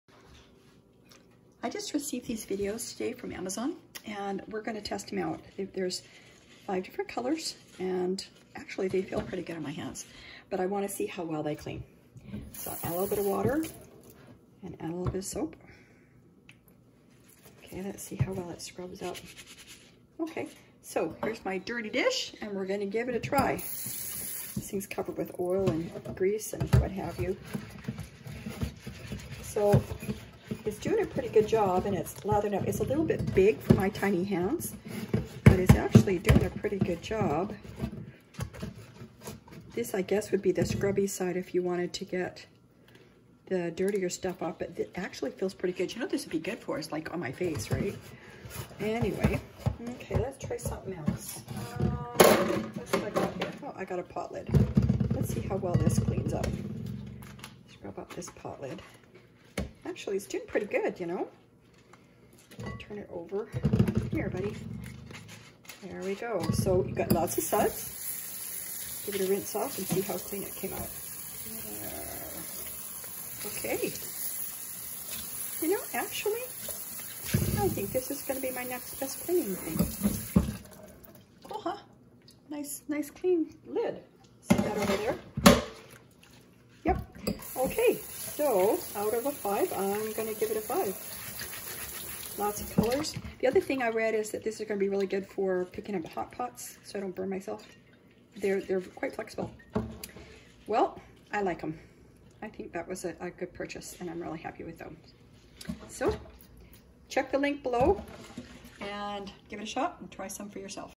JAGURDS set of 5 cleaning sponges for doing dishes (review)
I was very pleasantly surprised at how well these sponges did to clean dishes.  I usually put them in the dishwasher because its a pain to do dishes but with these brightly colored fun sponges that soap up well and are so fun to use, I may be doing more dishes by hand. They feel so good on my skin that I may swipe one and use it on my face in the bathroom.  Get some and try them yourself, you'll love them!  I got mine here: